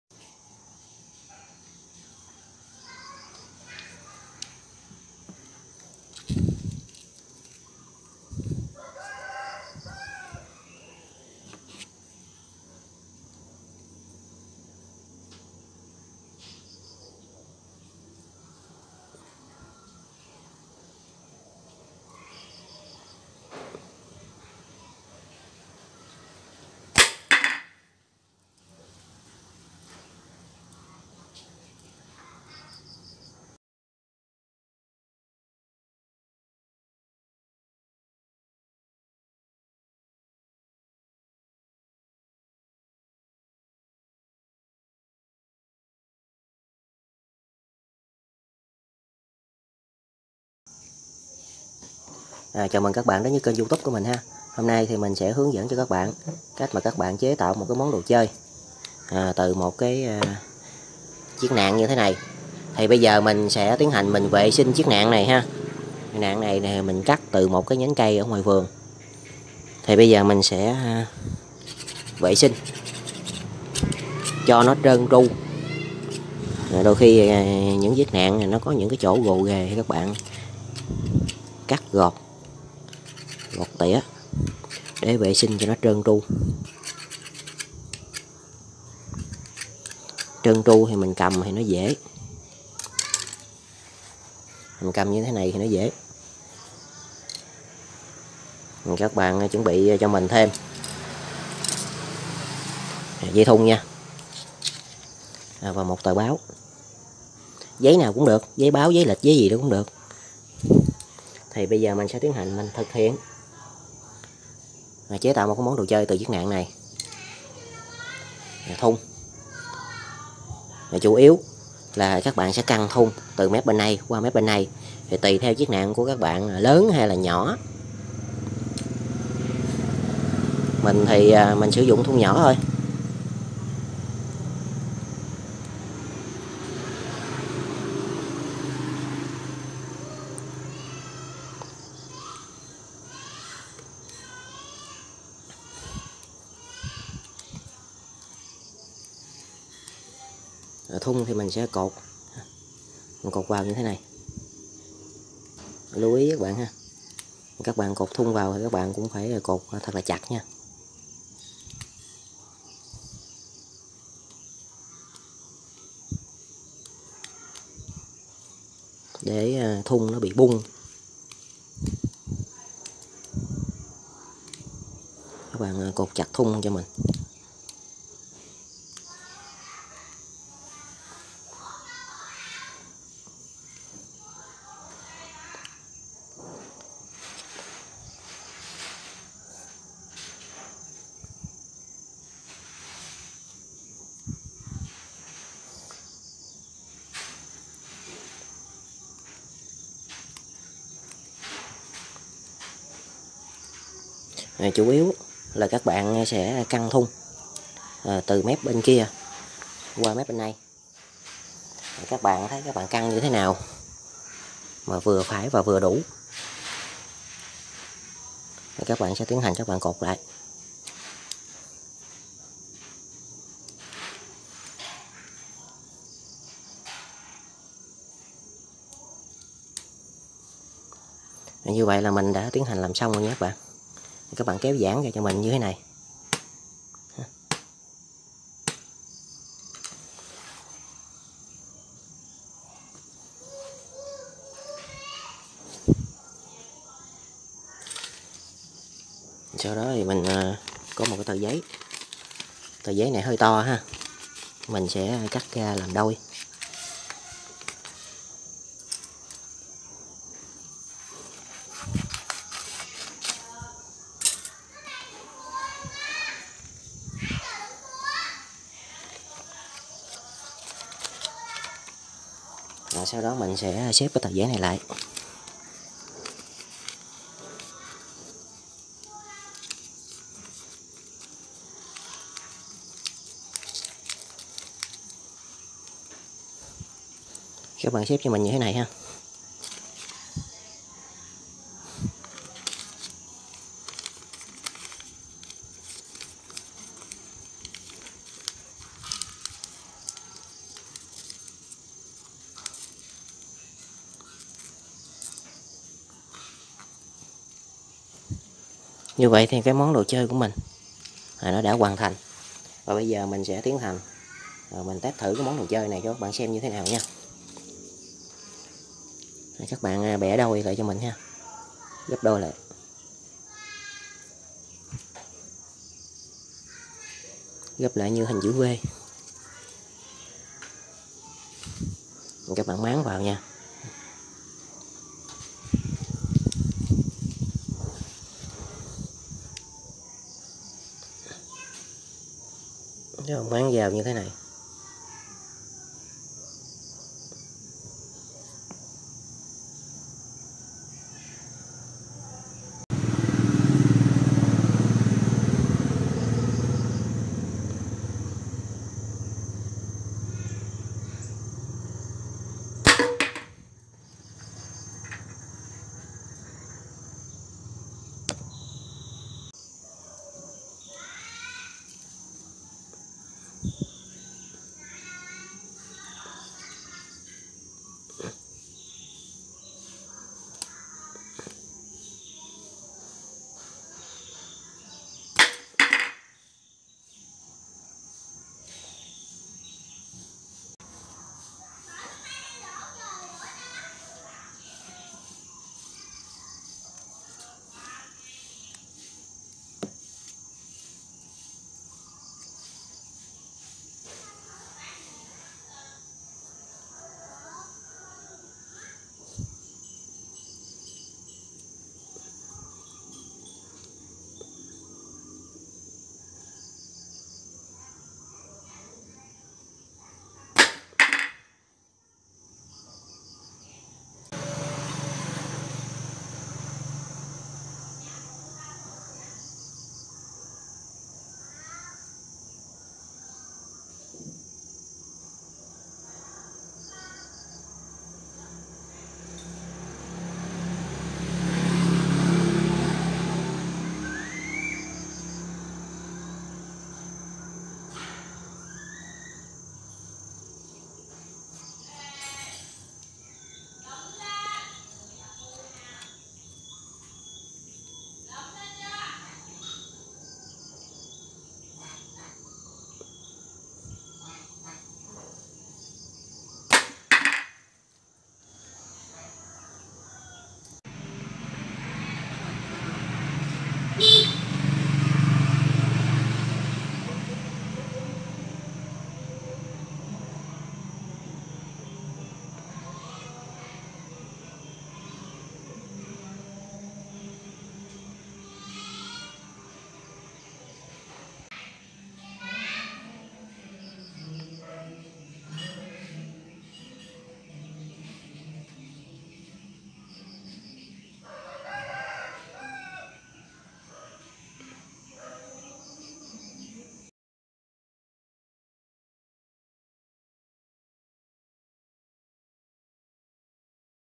CHẾ TẠO ĐỒ CHƠI TỪ NẠN CÂY, MAKING TOYS FROM TREES
Hướng dẫn chế tạo đồ chơi từ nạn cây
Đăng ký ủng hộ kênh mình nhé
Instructions for making toys from tree disasters